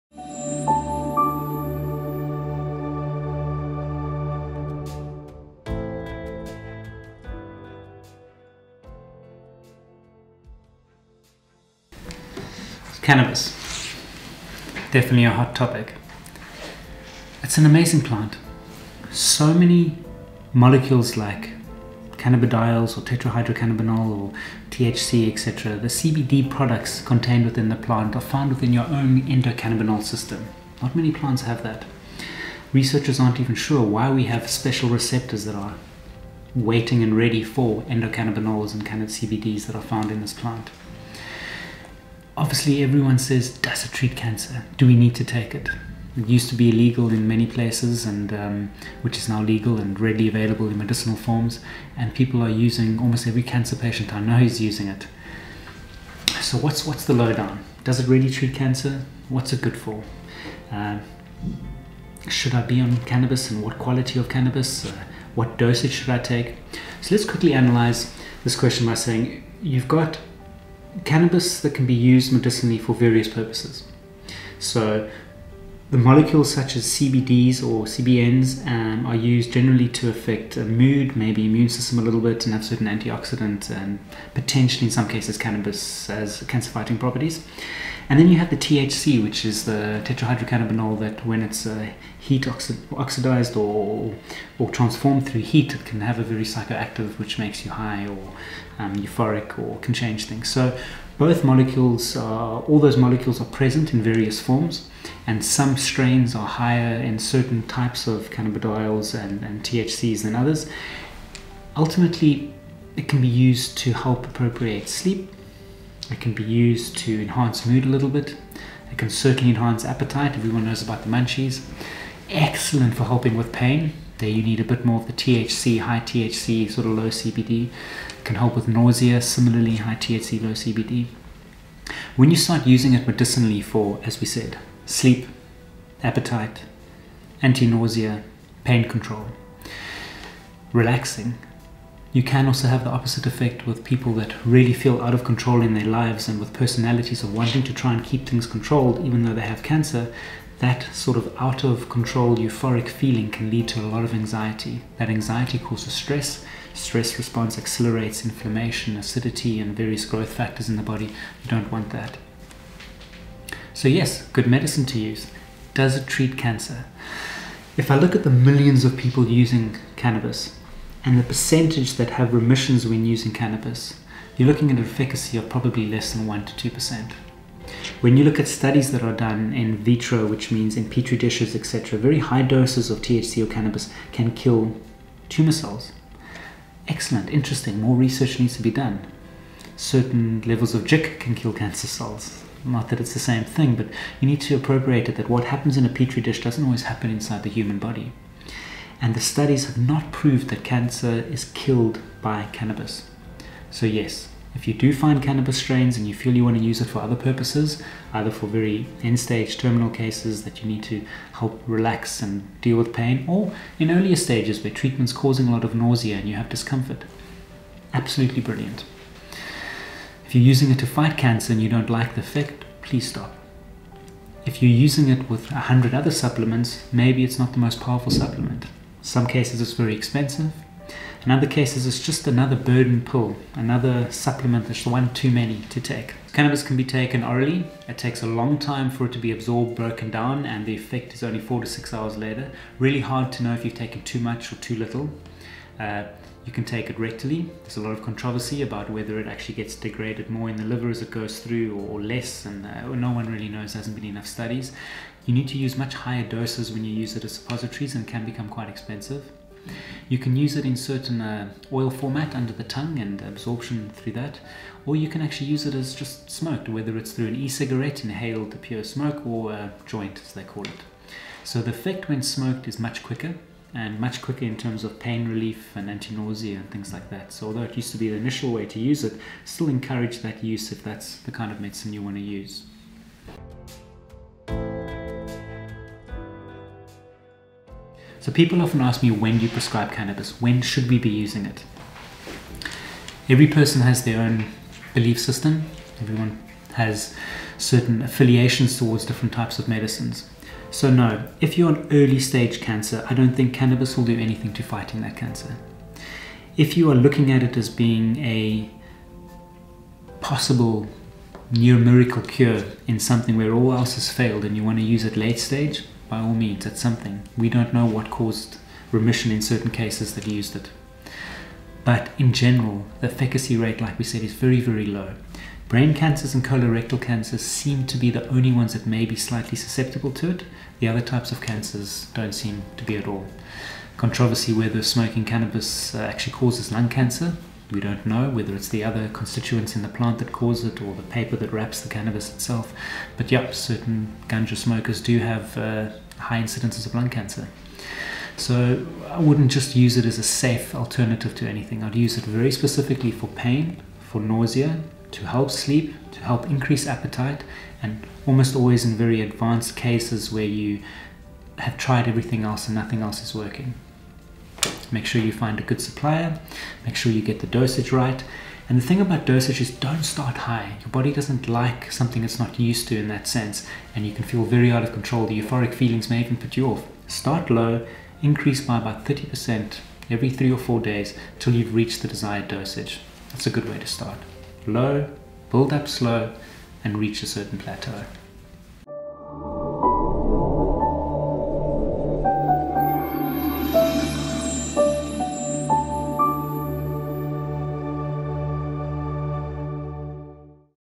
Is Cannabis Effective In Cancer Treatment? (Does it kill cancer cells?)
Some people claim it is a cure-all. Scientists say it has only a 2% success rate with cancer patients.
Knowing how to use medicinal cannabis to alleviate pain and nausea symptoms, may help your cancer journey. But for others, it might not be as effective and actually cause anxiety. And that can cause tumor cells to grow under the stress.

In this video, we discuss this hot topic, covering all the bases in cancer care. It's not as easy as just lighting up a joint. Knowing what level and balance of CBD and THC, is necessary for correct doses of an effective treatment.